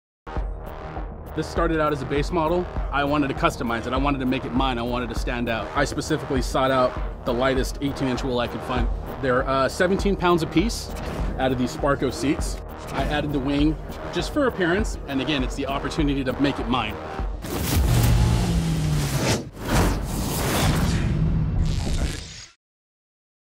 “Make It Mine” – Polaris Slingshot
We knew as soon as they hit the floors, the customs would follow. Check out how this owner made his Slingshot stand out .